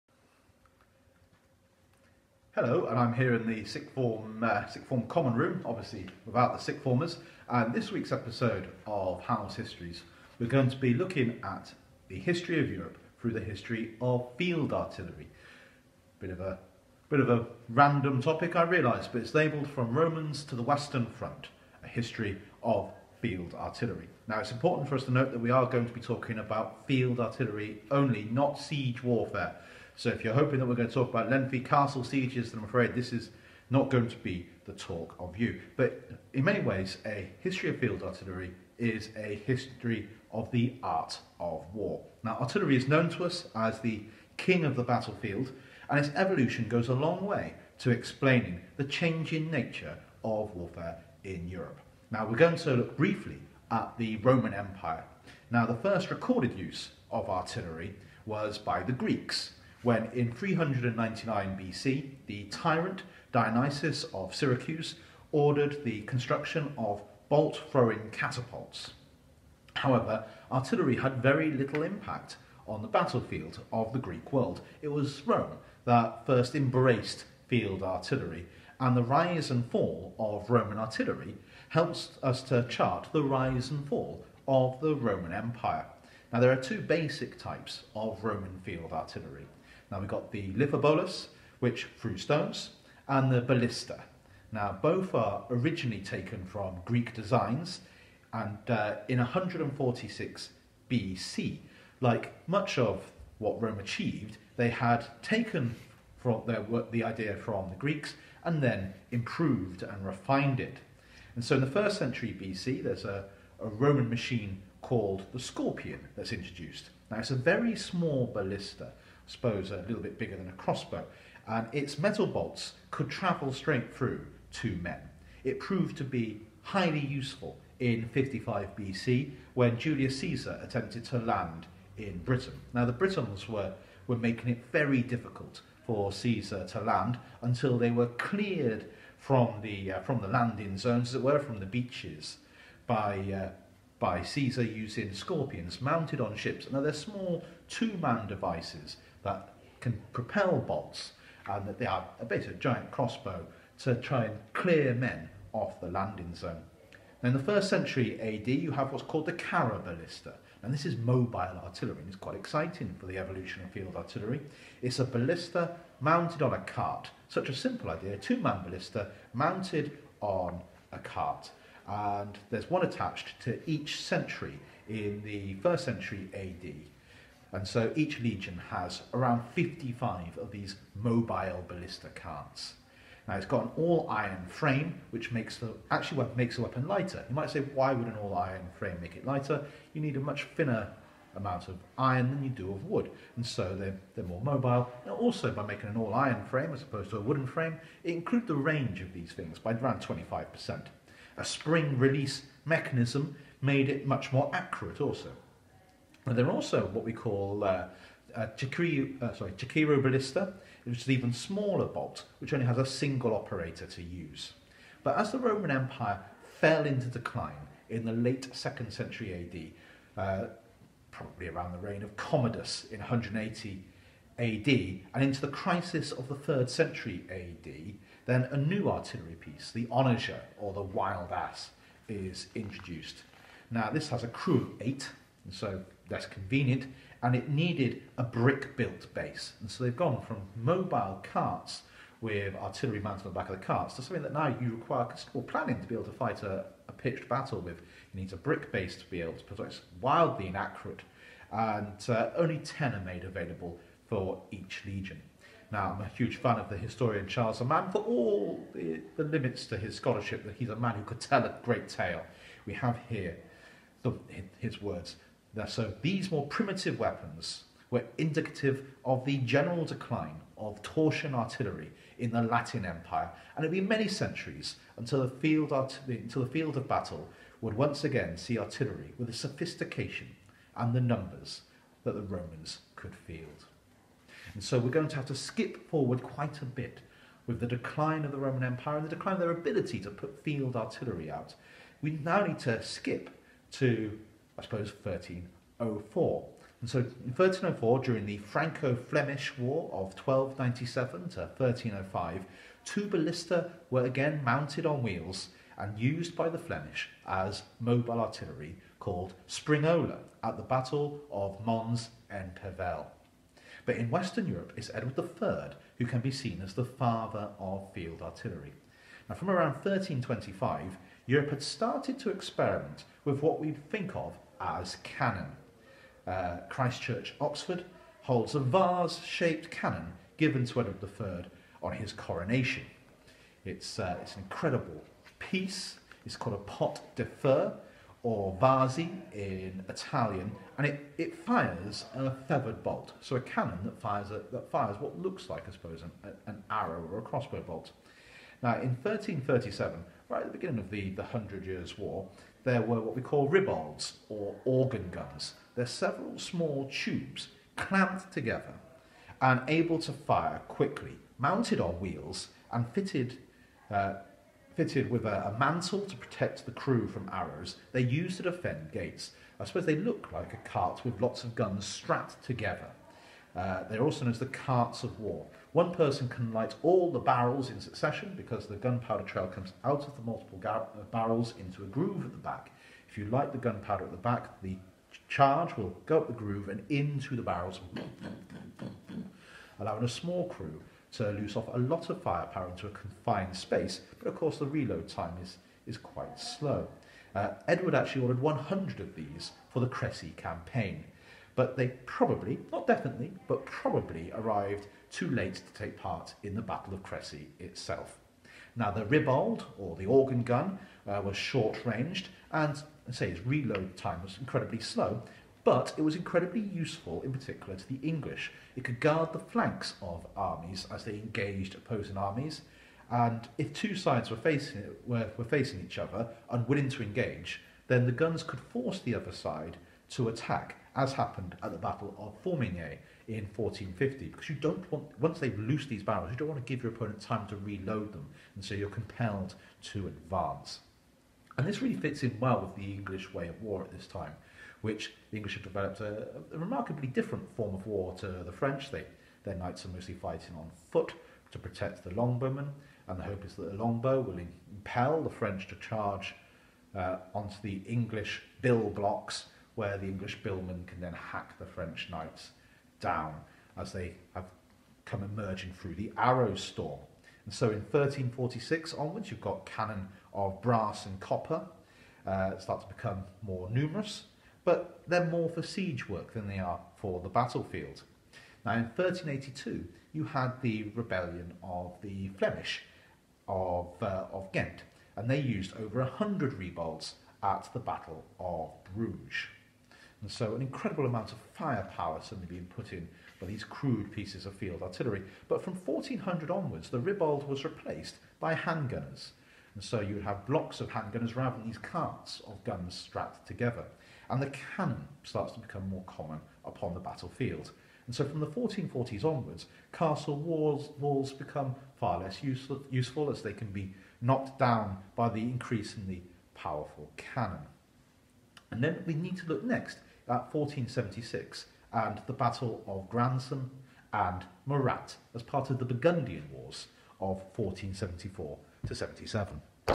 From the Romans to the Western Front, a brief History of the Evolution of Field Artillery 1
The Evolution of Field Artillery is the Evolution of warfare in the western world. Video 1/4